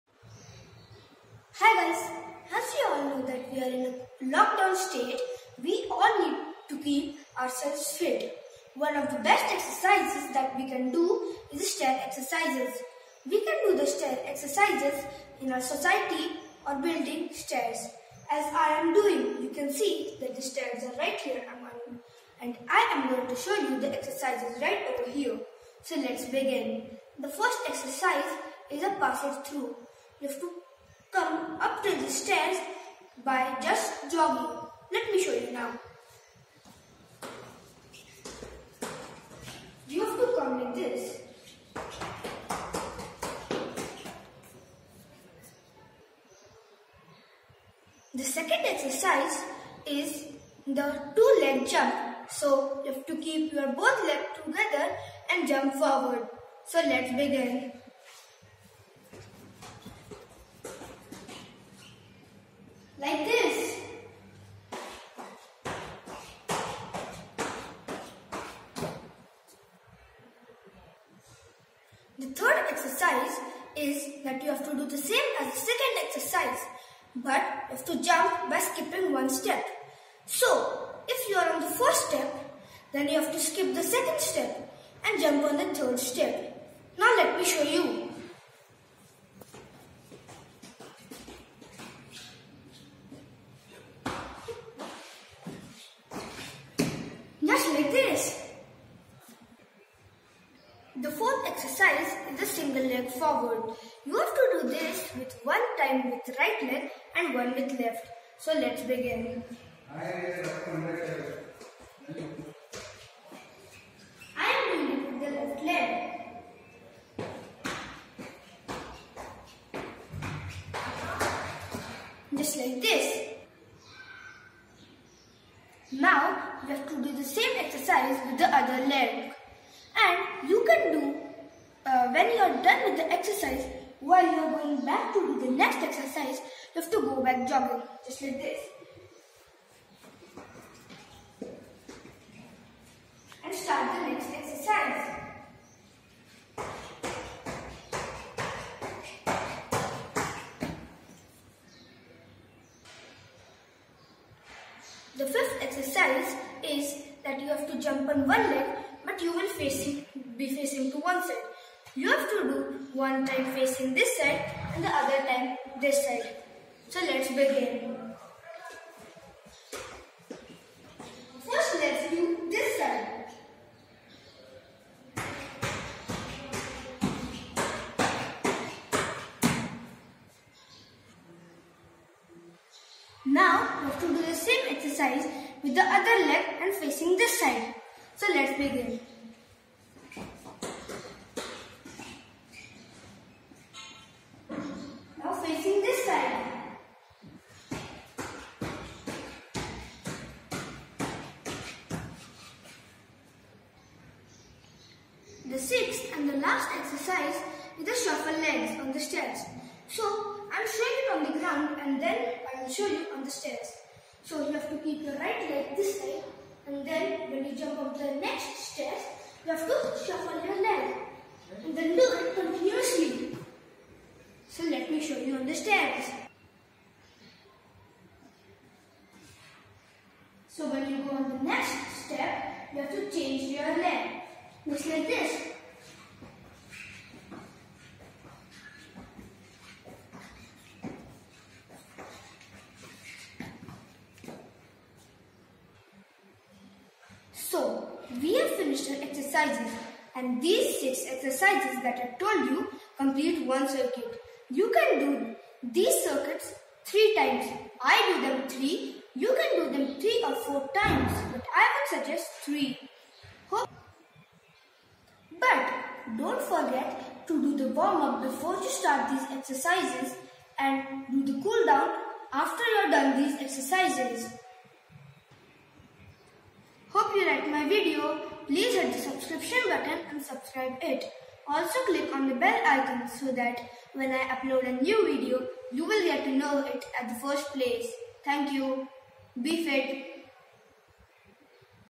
Steps exercises with Aarav
Its important to stay fit during lockdown. Learn Steps exercises with Aarav which are part of Dryland workouts for any skater or just to keep you fit. These exercises help strengthen your legs.